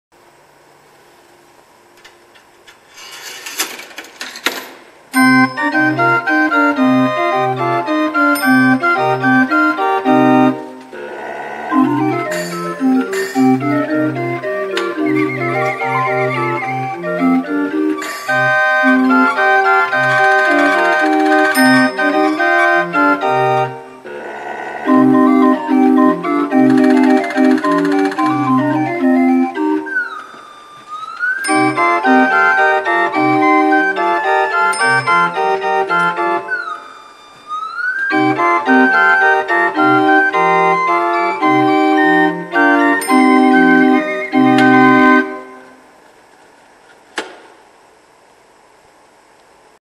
Orchestrion/Music box from the Schwarzwaldmuseum
Orchestrion/Musical box from the marvellous Schwarzwaldmuseum collection, in Triberg im Schwarzwald, Germany. Didn't play any more songs to save the euros for the big orchestrion, but there's another song from the same machine in this video